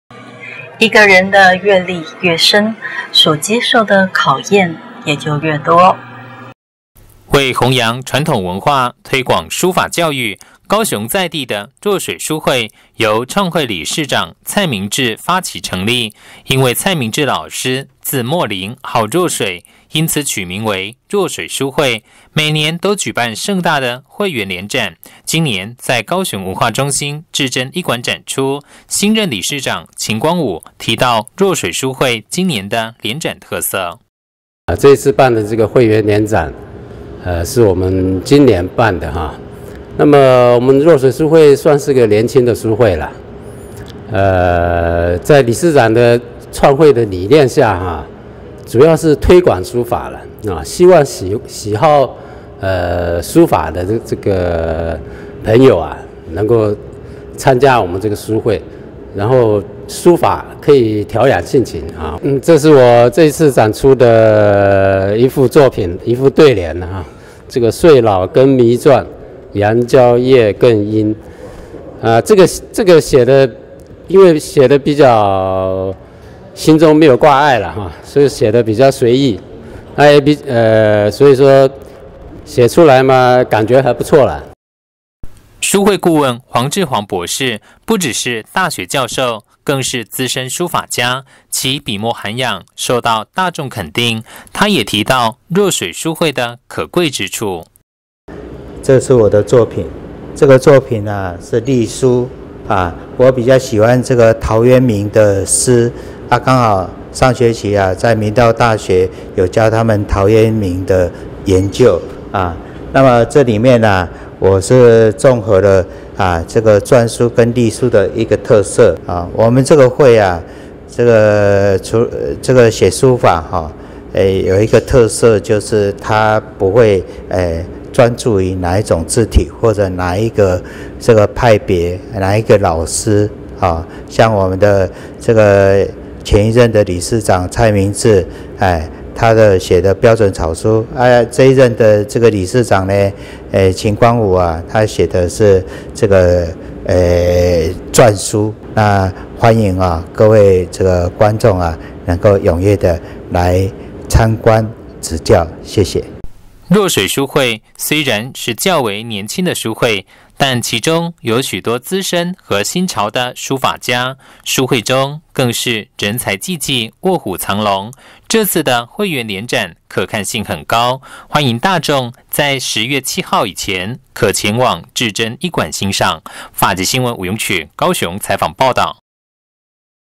法界新聞》20141002高雄市若水書會聯展
法界新聞》
受訪者：高雄市若水書會 理事長 秦光武

陽光越強，枝葉越顯得茂盛蔥郁。
以此形容人的閱歷越深，所受考驗越多，就越堅強。